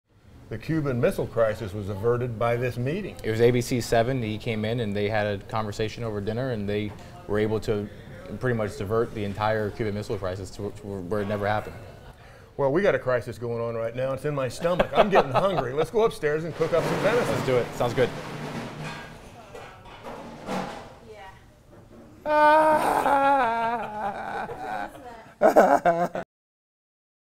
Outtakes: Occidental Grill w/Chef Matt Baker
Sometimes you get it on the first take... sometimes... you don't.

Tune in to Comcast SportsNet Saturday AM as Riggo tracks down deer from his hometown in Kansas to Wyoming, and then gets the assistance of Chef de Cuisine Matt Baker, of The Occidental Grill in DC, to take this venison to the next level. Two fantastic recipes on this week's show that you don't want to miss!